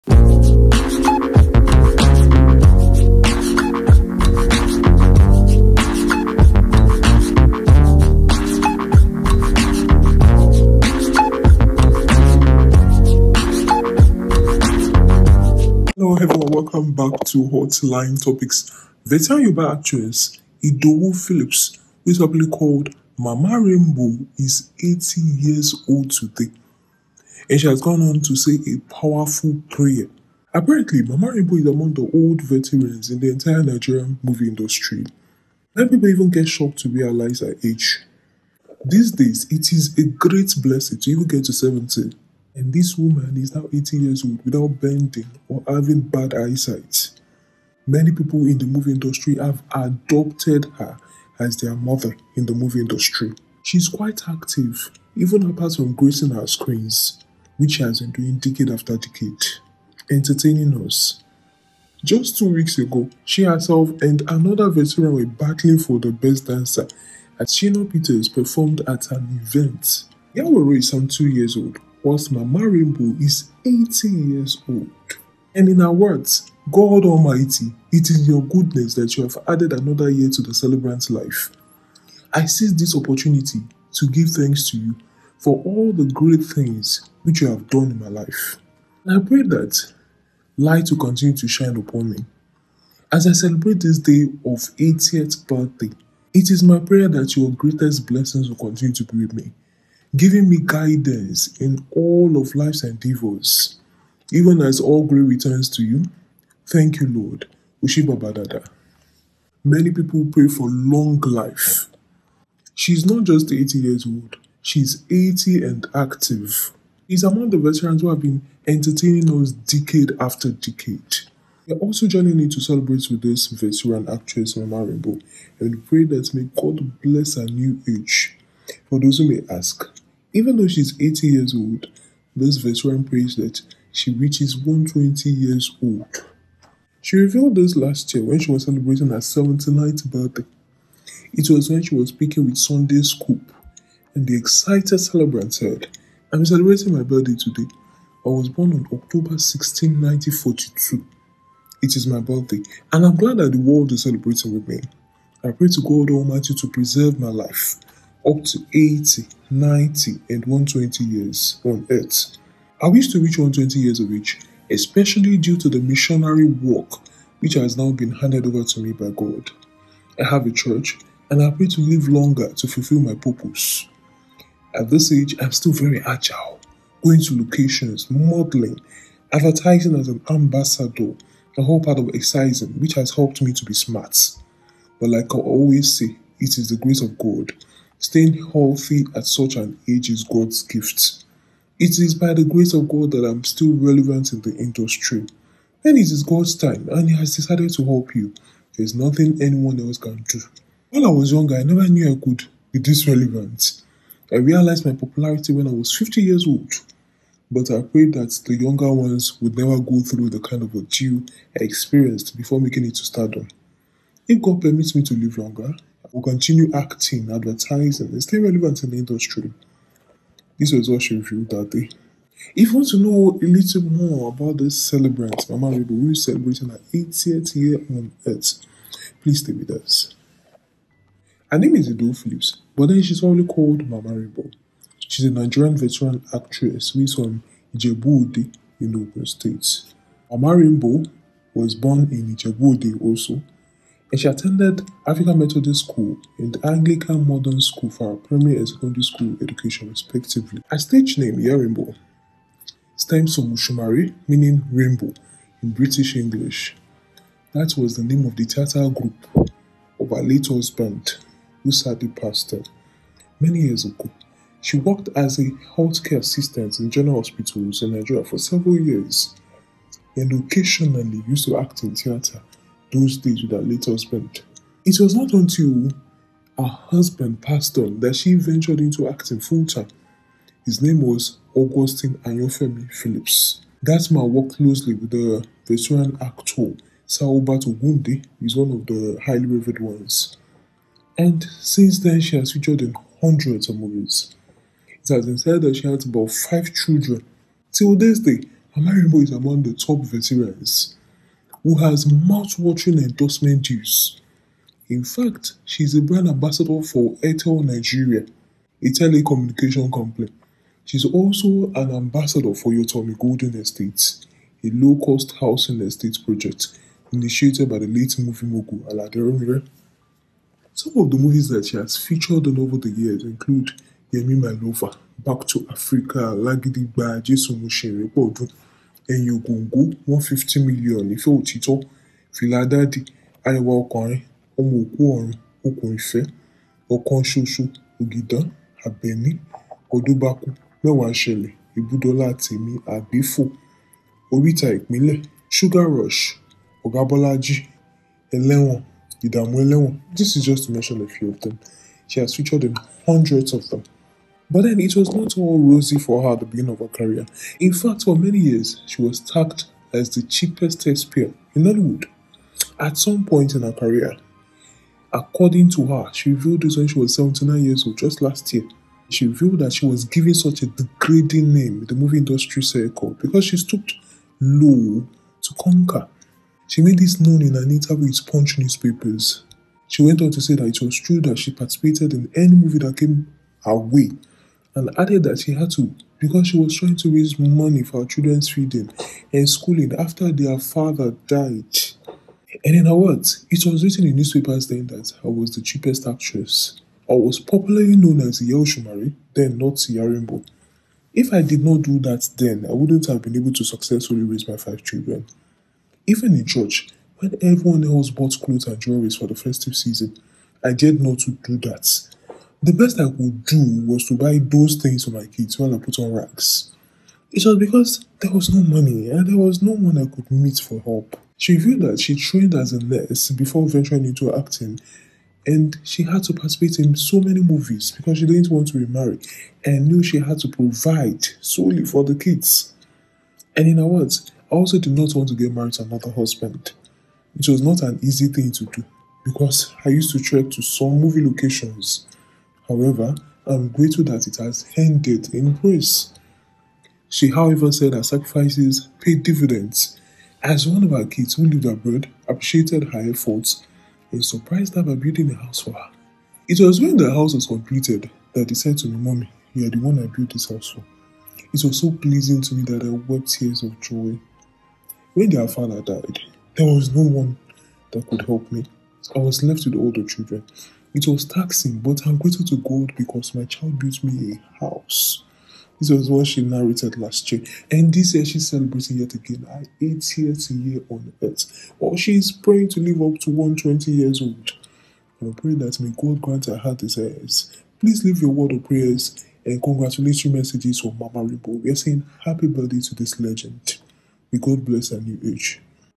Idowu Philips Excitedly Celebrates Her 80th Birthday, Asks God To Bless Her With 120 Years And….
Veteran Yoruba Actress Idowu Philips AKA Iya Rainbow/Mama Rainbow Is 80 Years Old Today, Says A Powerful Prayer